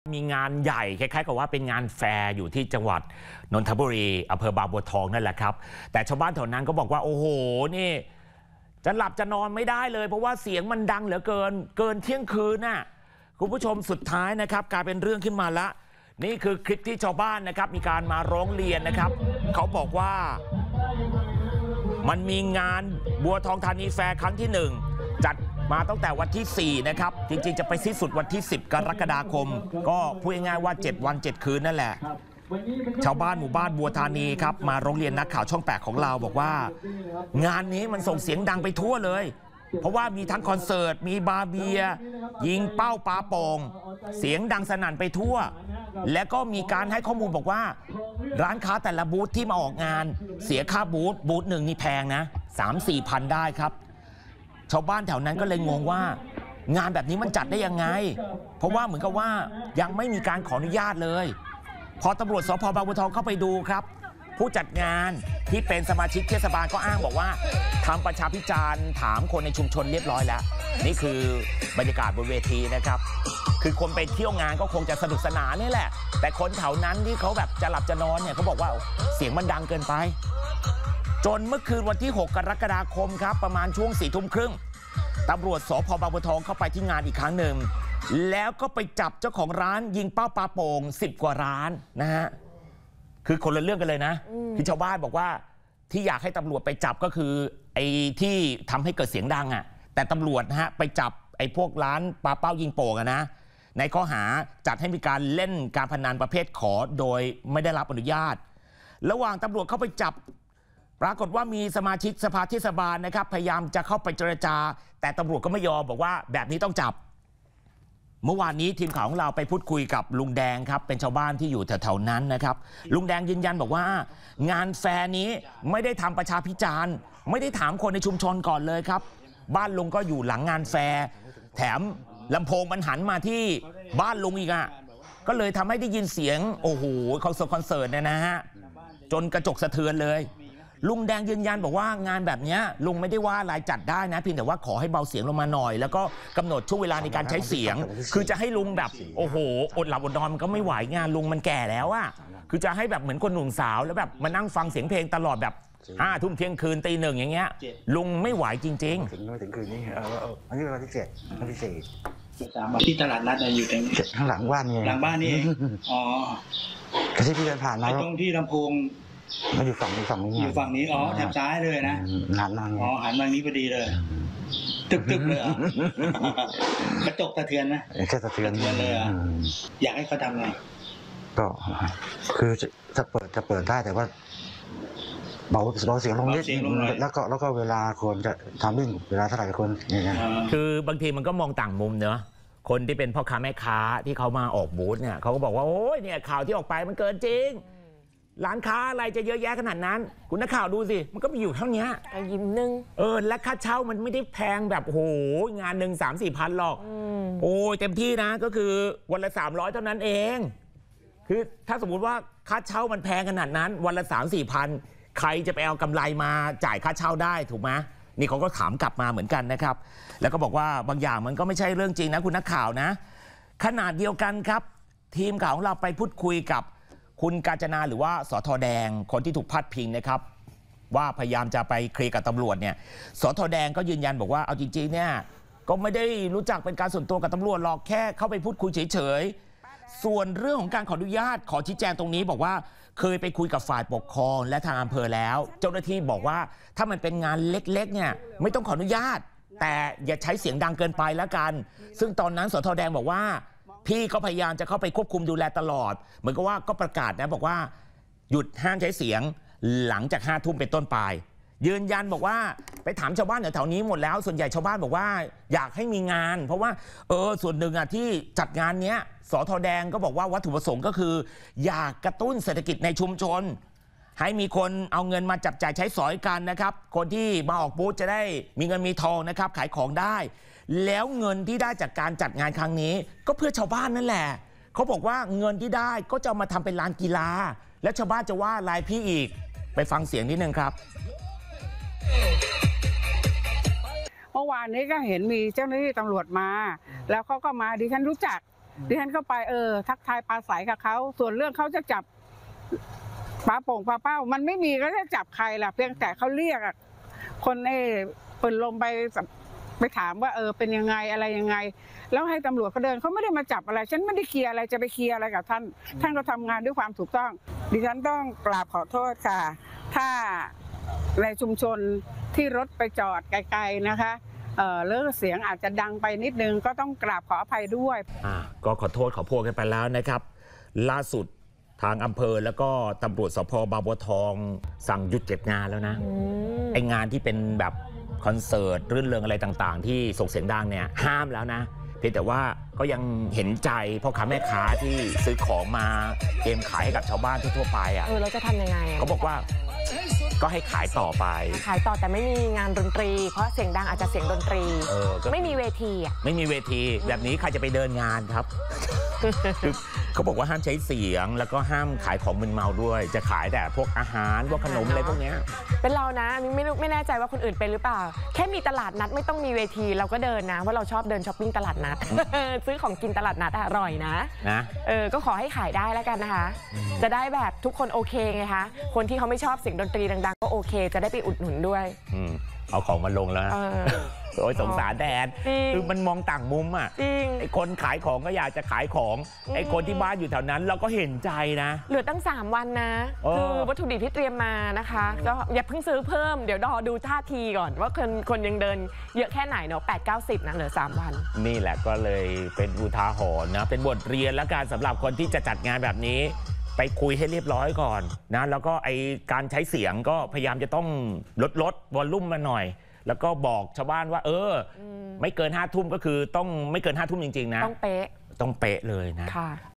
ชาวบ้านเดือดร้อน! งานแฟร์เล่นคอนเสิร์ตดังลั่นหมู่บ้าน | 10 ก.ค.67 | คุยข่าวเช้าช่อง8
งานแฟร์กลางหมู่บ้าน เล่นคอนเสิร์ต-เปิดเพลงยันเที่ยงคืน จนชาวบ้านไม่ได้หลับได้นอน